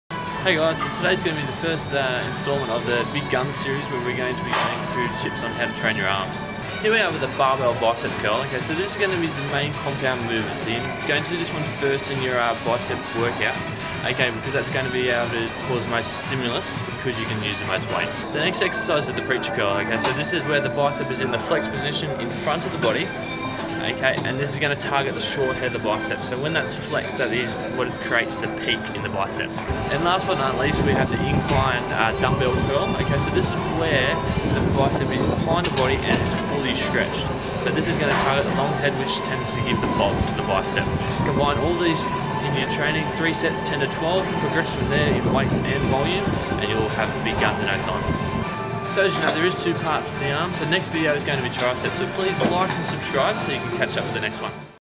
Growing Big Guns | Episode 1
You want to get the most of your arm training and grow massive guns? You need to hit your arms from all the right angles.

Here I demonstrate how to do 3 different exercises and how to apply them to your training.